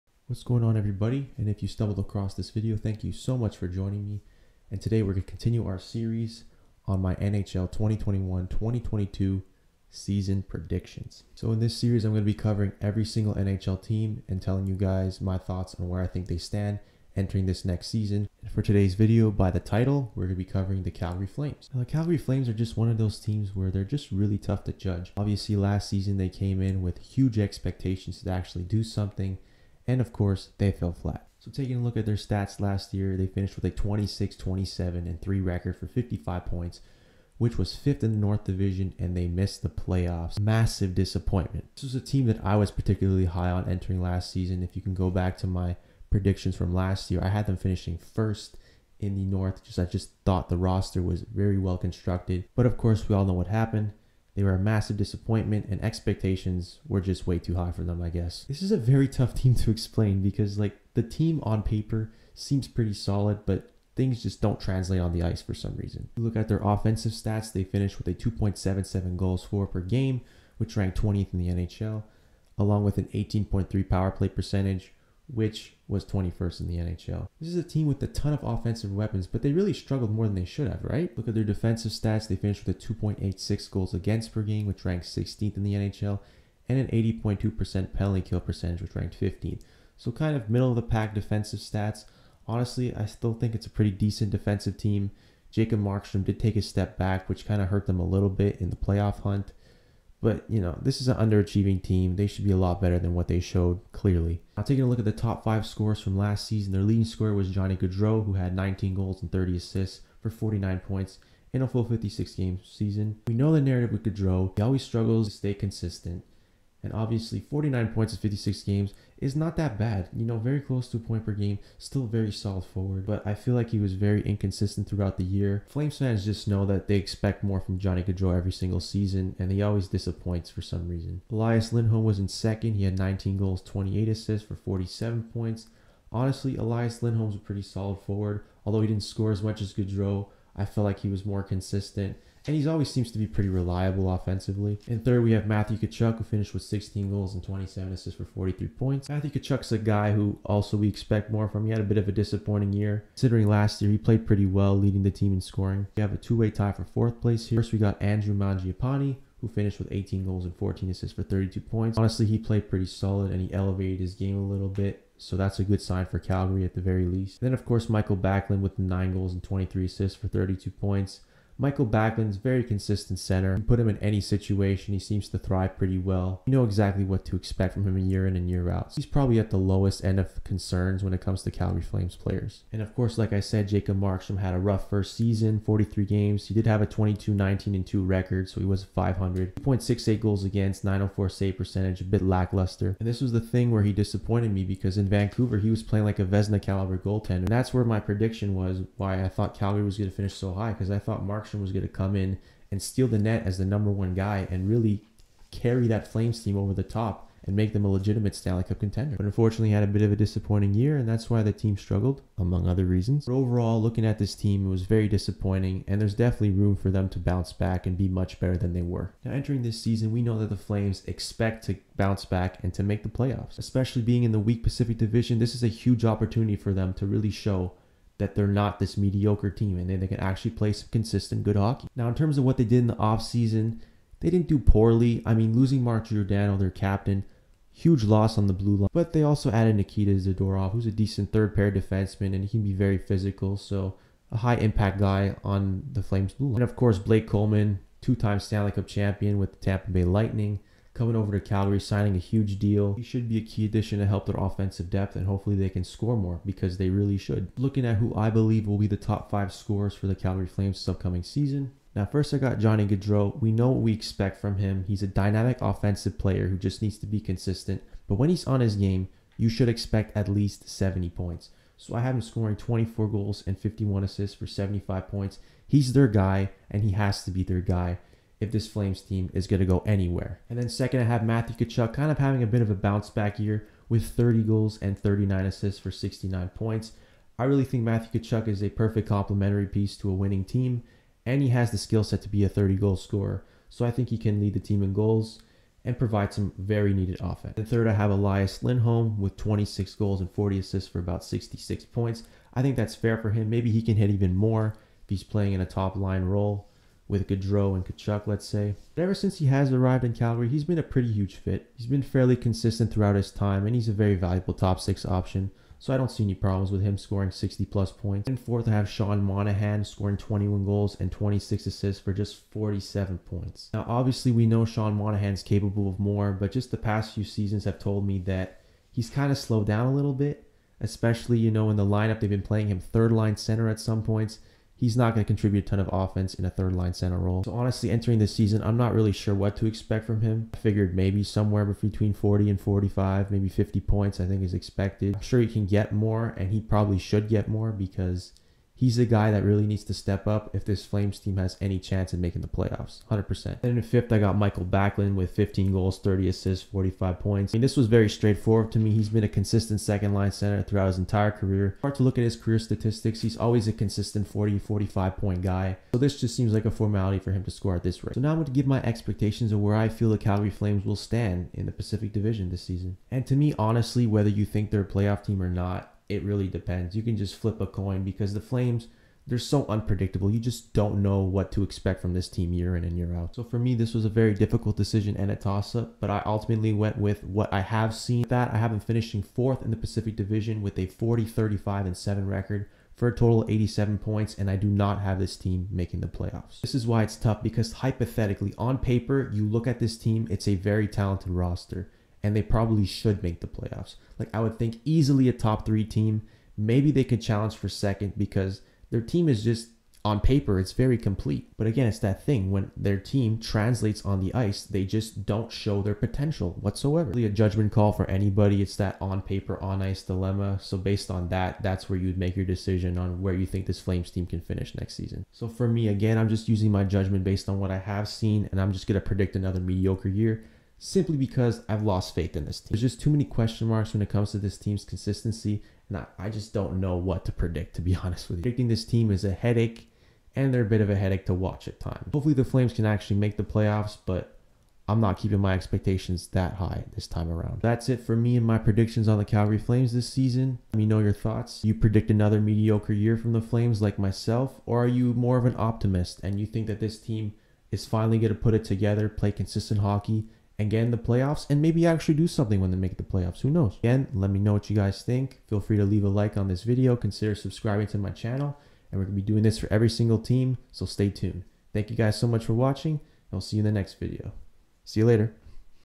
Calgary Flames 2021-2022 Season Preview
Continuing with my NHL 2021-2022 season predictions, this video will be covering my thoughts on the Calgary Flames where I think they stand entering this upcoming season, and where I expect them to finish in the Pacific division. Can the Flames finally get out of their inconsistent ways and prove that they can be a legitimate Stanley Cup contender, or will they disappoint yet again and fail to meet expectations?